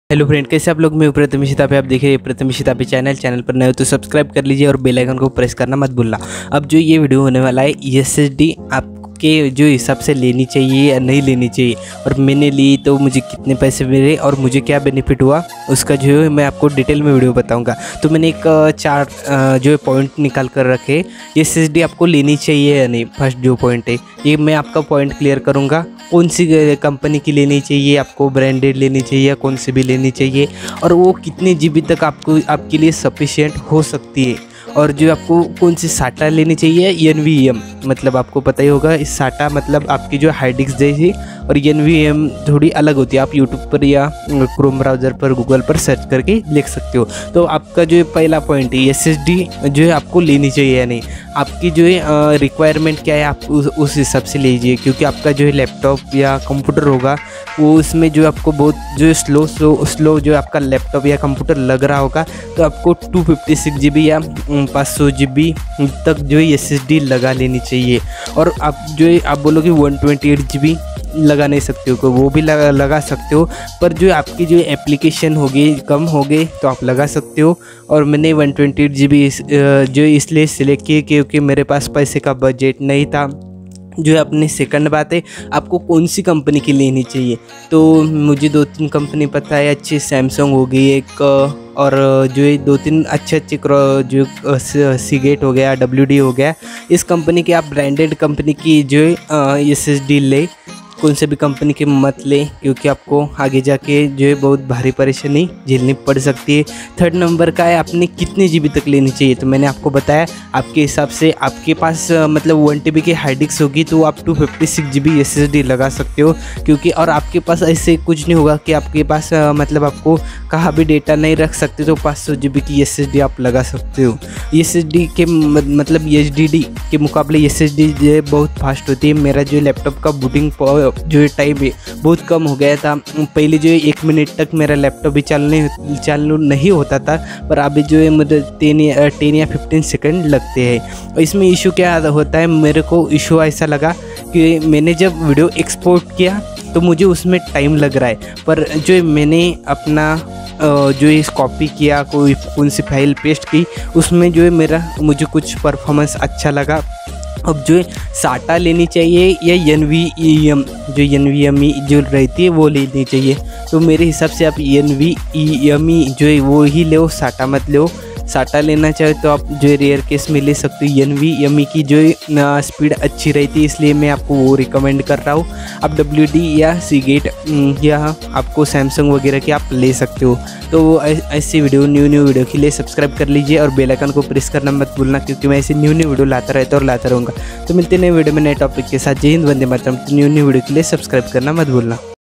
All About SSD | 128GB vs 256GB vs 512GB SSD Kitna lene Chahie? How Much SSD is Enough for Laptop
➤Guys in this video we will see-All About SSD | 128GB vs 256GB vs 512GB SSD Kitna lene Chahie? How Much SSD is Enough for Laptop

0:00 Intro
0:50 SSD लेनी चाहीए या नही
1:25 कोनसी company की लेनी चाहीए
1:46 कितने GB की लेनी चाहीऐ
2:36 SATA या NVMe
3:00 End